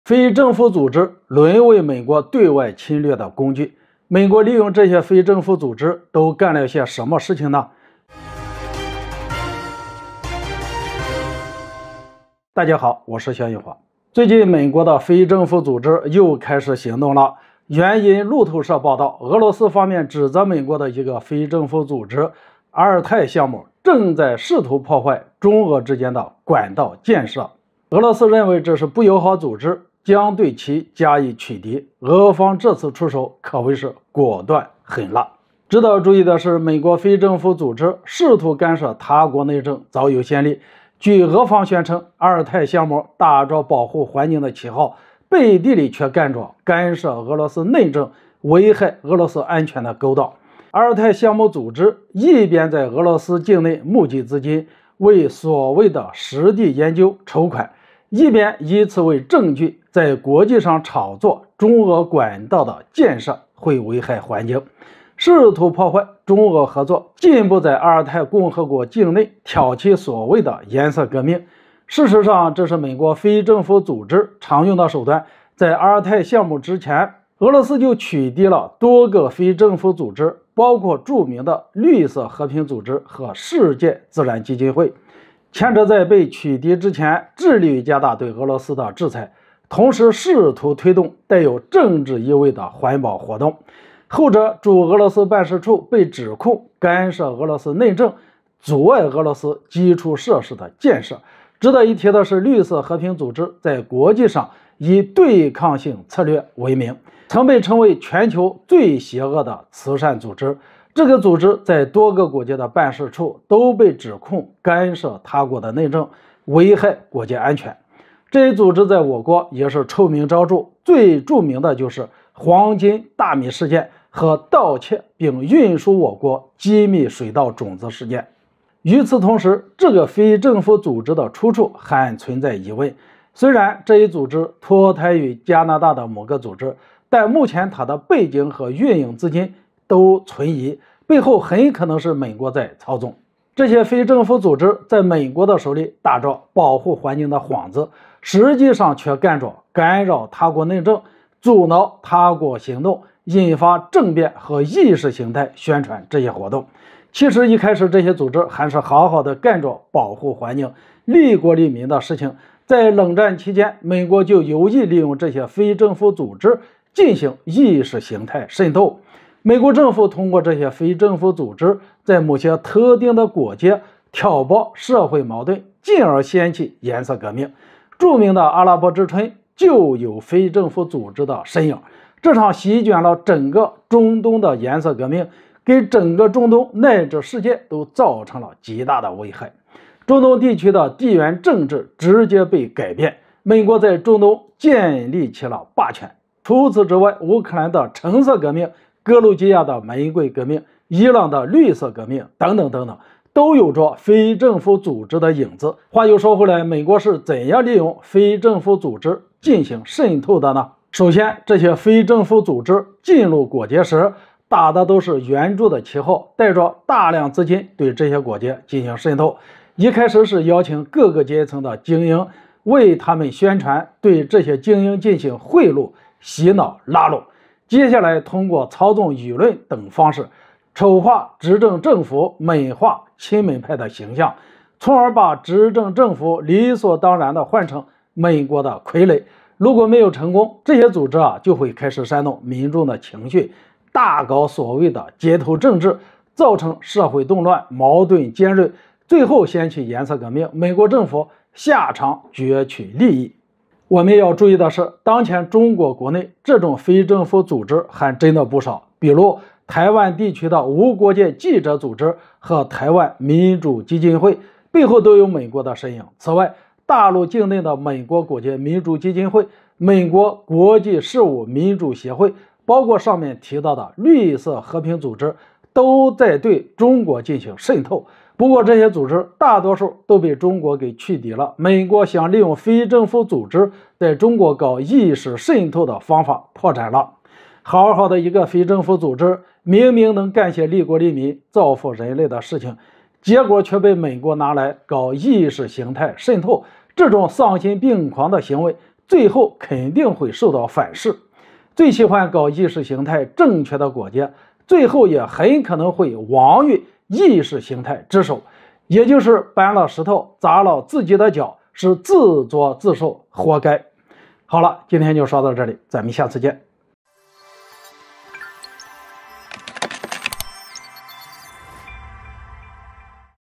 一個美國組織將被取締，破壞中俄管道建設項目，俄羅斯出手狠辣【肖允华】
非政府組織淪為美國對外侵略的工具，美國利用這些非政府組織都乾了些什麼？
一個美國組織將被取締，破壞中俄管道建設項目，俄羅斯出手狠辣